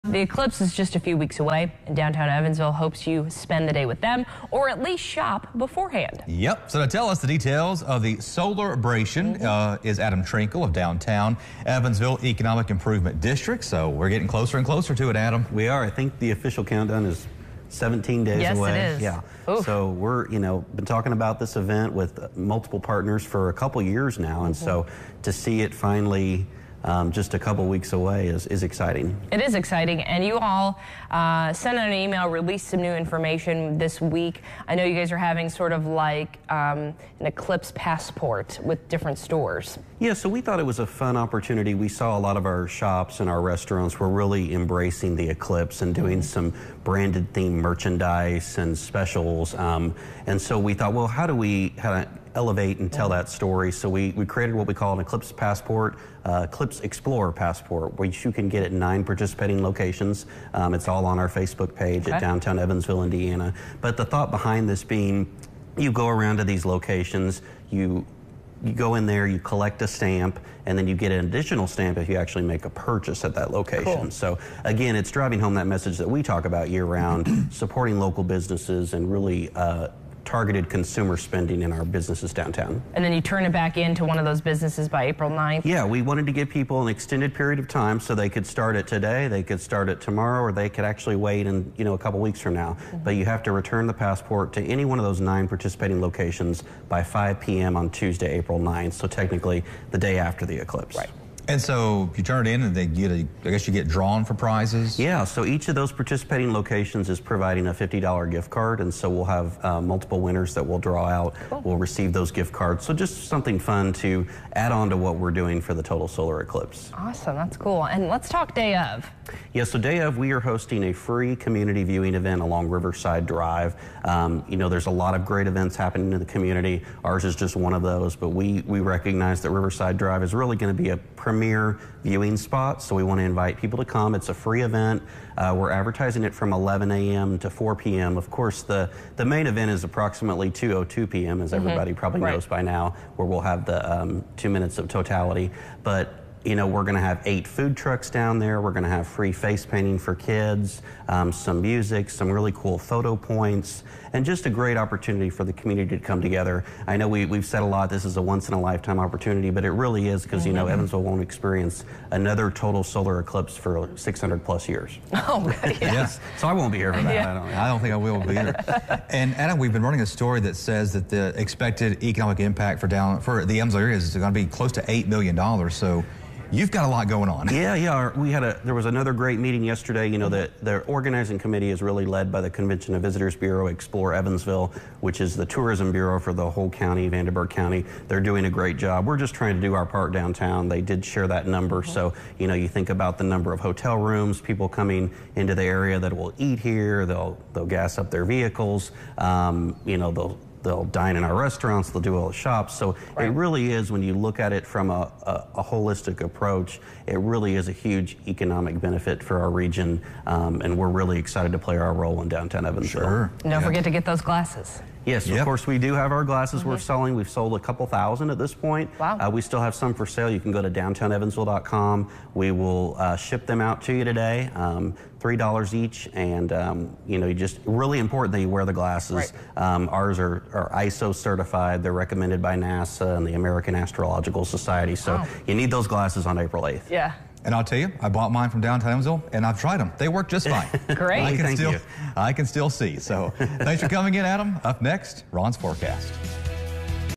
Downtown Evansville details plan for Total Solar Eclipse
The total solar eclipse is only a few weeks away, and Downtown Evansville hopes that you spend the day with them.
The Total Solar Eclipse celebration in Downtown Evansville will take place on April 8 from 11:00 a.m. to 4:00 p.m. on Riverside Drive and will feature food trucks, face painting, music, games and photo points for visitors.
A new program called the Eclipse Explorer Passport will also take place. Visitors can visit downtown businesses and collect stamps to turn in for possible gift cards. Stamps must be turned in by April 9.